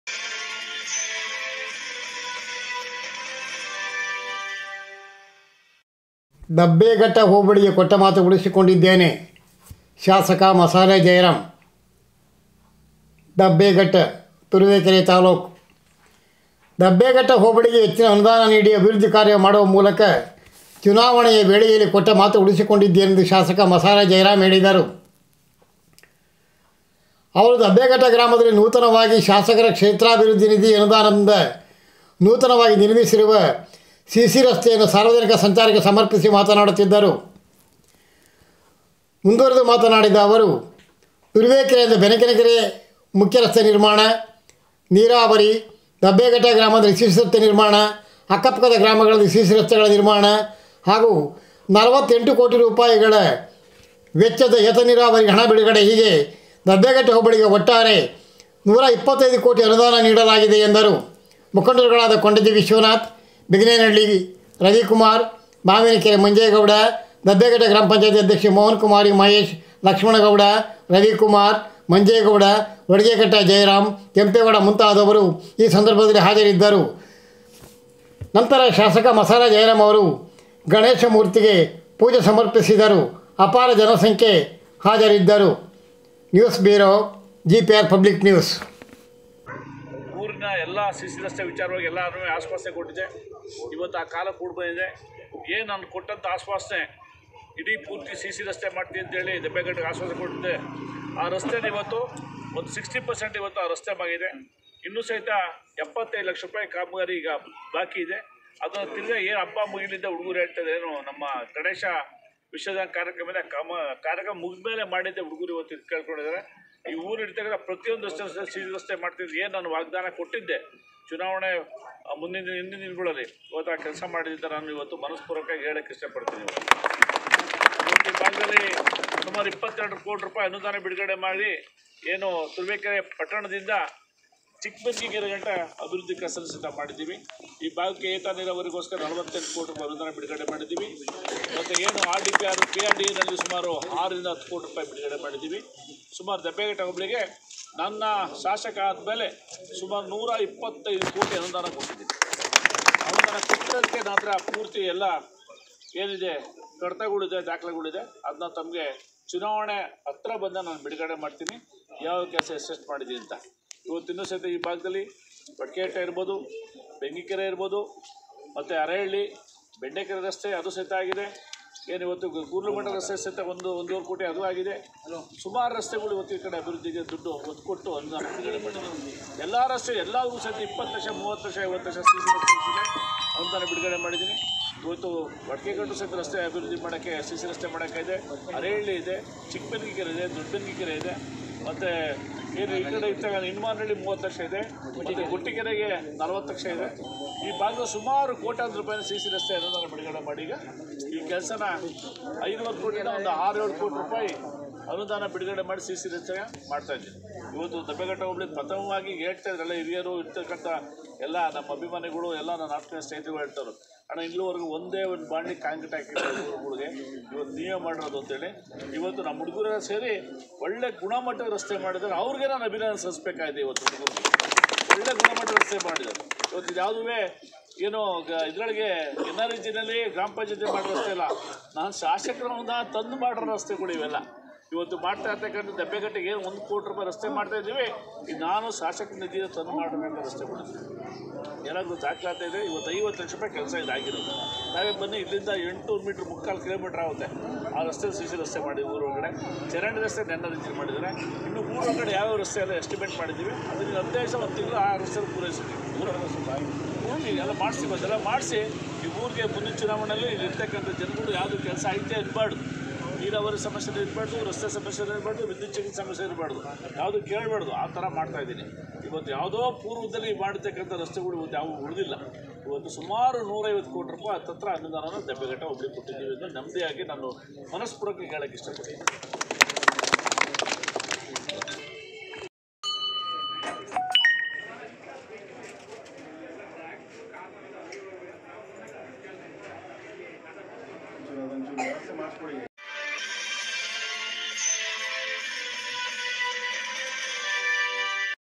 ಮಾತು ಉಳಿಸಿಕೊಂಡಿದ್ದೇನೆ -ಶಾಸಕ ಮಸಾಲೆ ಜಯರಾಮ್.# turuvekere# @ gpr public news
ದಬ್ಬೇಘಟ್ಟ ಹೋಬಳಿಗಿ 125 ಕೋಟಿ ಅನುದಾನ ನೀಡಿ ಅಭಿವೃದ್ಧಿ ಕಾರ್ಯ ಮಾಡುತ್ತಿರುವ ನಾನು ಕೊಟ್ಟ ಮಾತು ಉಳಿಸಿಕೊಂಡಿದ್ದೇನೆ ಎಂದು ಶಾಸಕ ಮಸಾಲೆ ಜಯರಾಮ್ ಹೇಳಿದರು. ದಬ್ಬೆಘಟ್ಟದಲ್ಲಿ ನೂತನವಾಗಿ  ನಿರ್ಮಿಸಿರುವ ಸಿಸಿ ರಸ್ತೆಯನ್ನು ಸಾರ್ವಜನಿಕ ಬಳಕೆಗೆ ಅ ನುವು ಮಾಡಿಕೊಟ್ಟು ಅವರು ಮಾತನಾಡುತ್ತಿದ್ದರು.